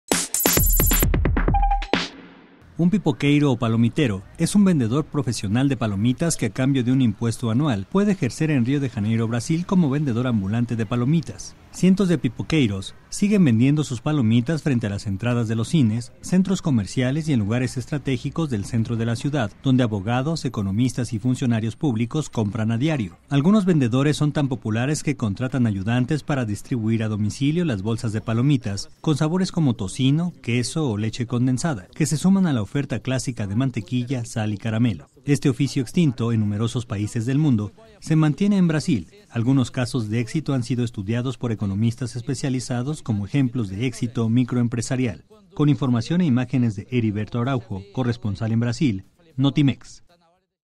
''Pipoqueiros'', un oficio casi extinto en el mundo que sobrevive en Brasil
Los Pipoqueiros, extintos en numerosos países del mundo, se mantienen ejerciendo en Brasil, con casos de éxito estudiados por economistas.